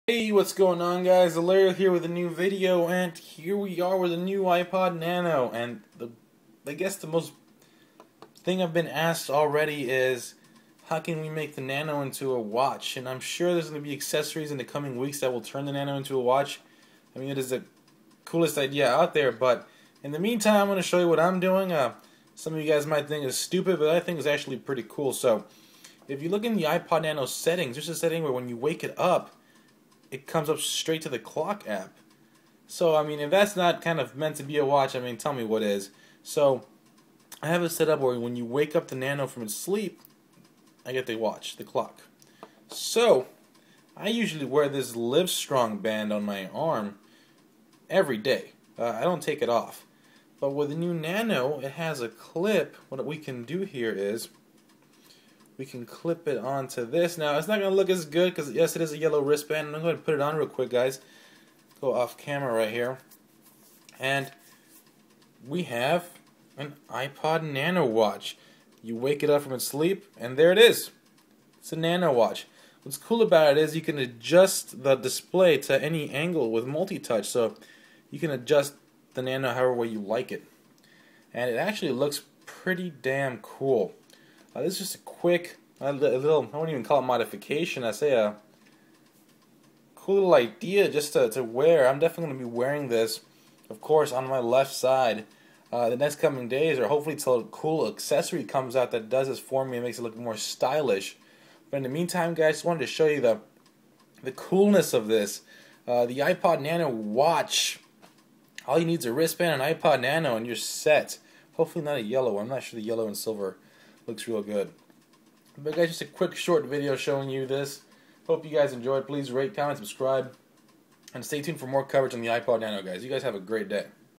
iPod Nano (2010) Watch
Quick idea how to turn you're iPod Nano 6th gen into a watch.